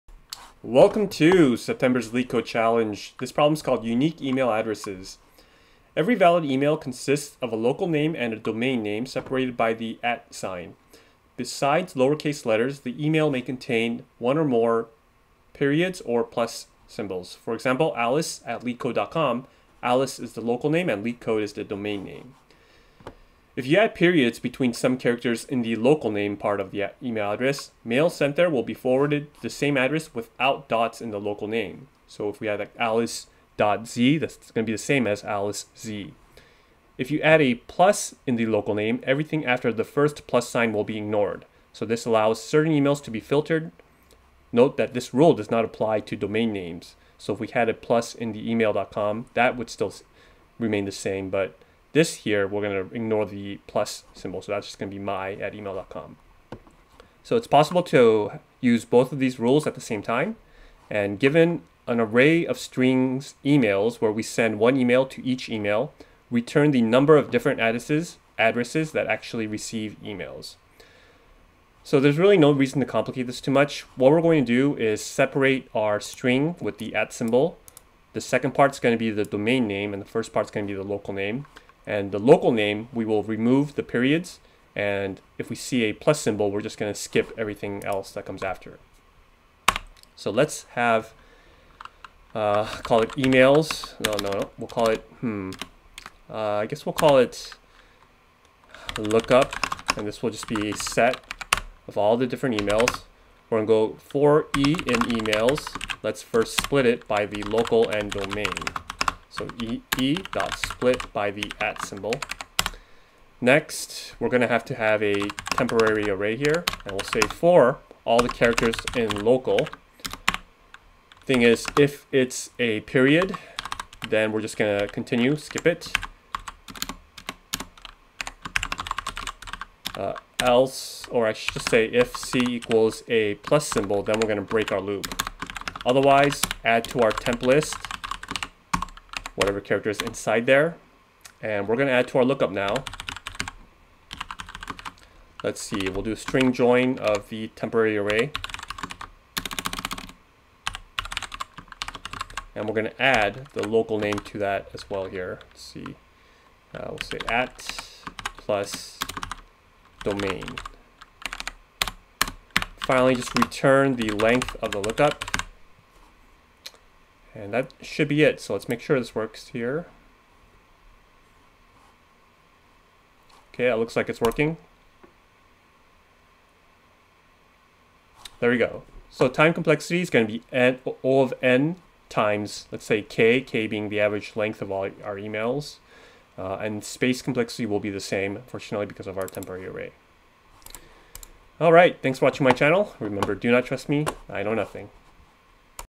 Leetcode - Unique Email Addresses (Python)
September 2021 Leetcode Challenge
Leetcode - Unique Email Addresses #929
Difficulty: Easy